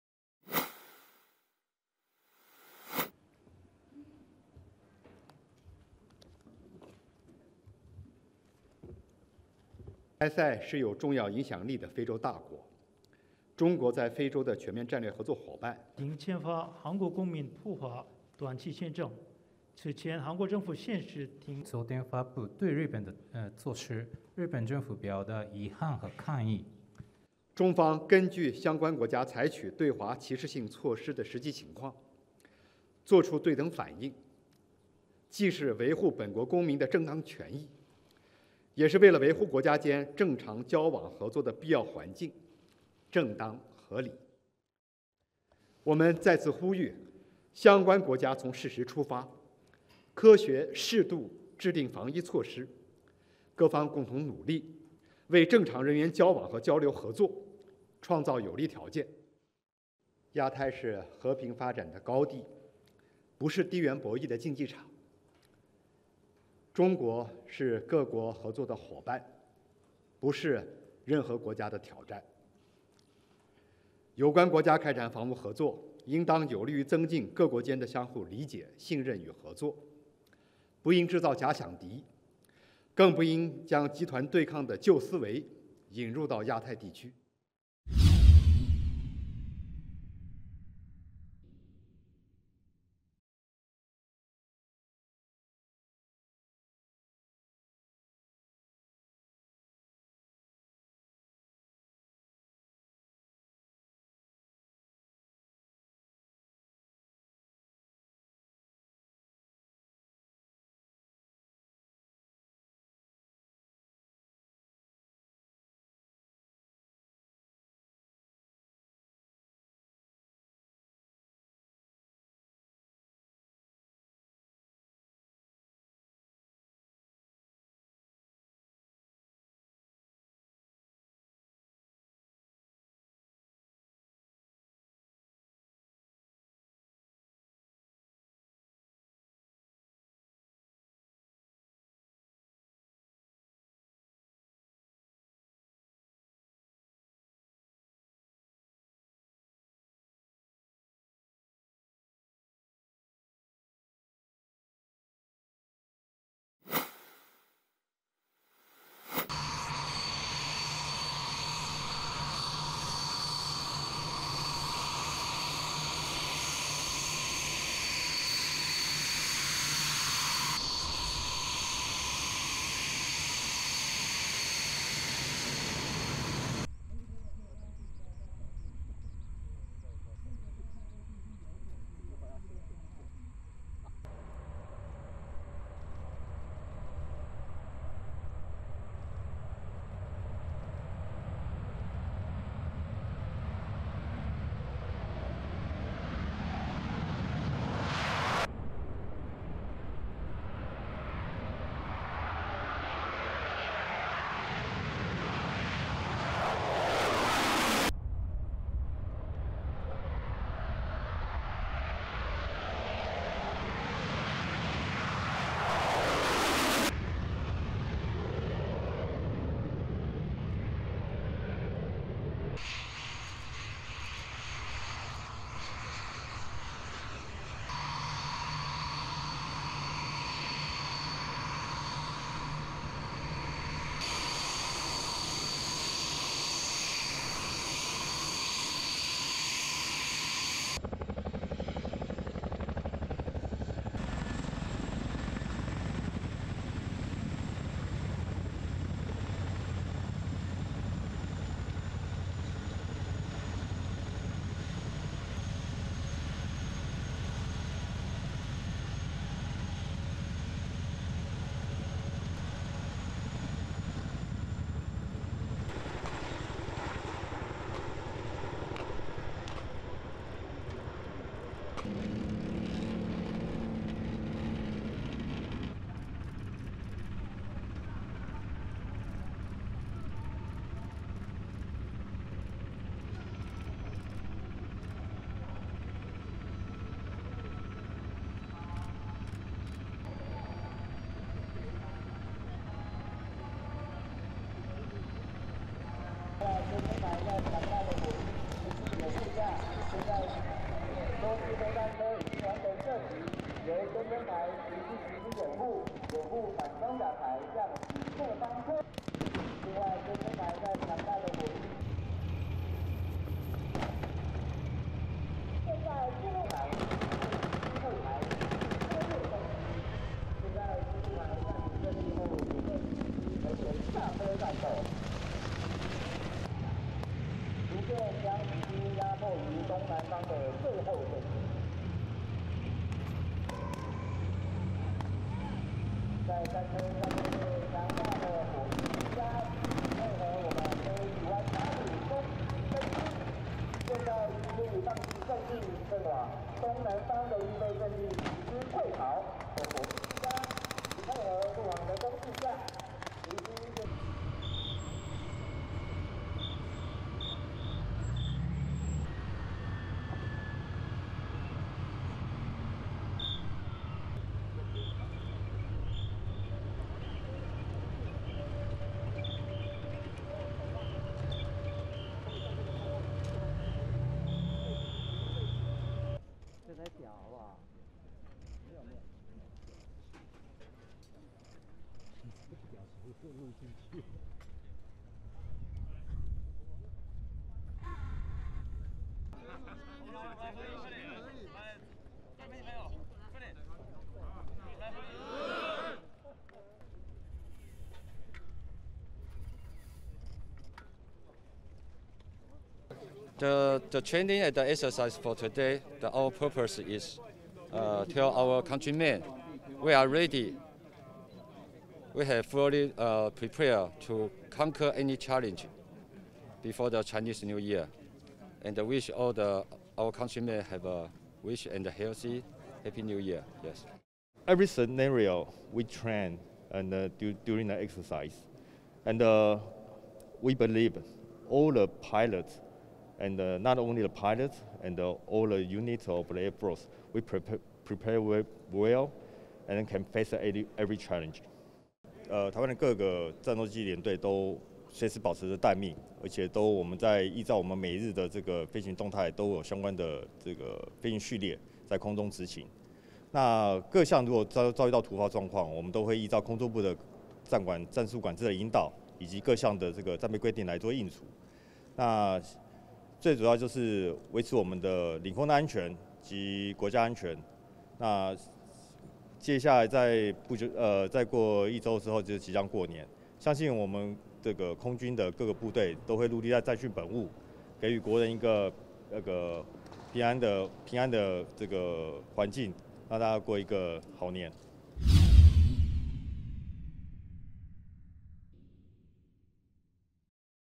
Taiwan carries out military exercises ahead of Lunar New Year
Taiwanese military shows its battle readiness ahead of the Lunar New Year as airforce fighter jets scramble to intercept enemy aircrafts and army soldiers repel an enemy attack.

The Lunar New Year, also known as the Spring Festival, begins on January 22 and is the most important holiday celebrated by Chinese communities worldwide.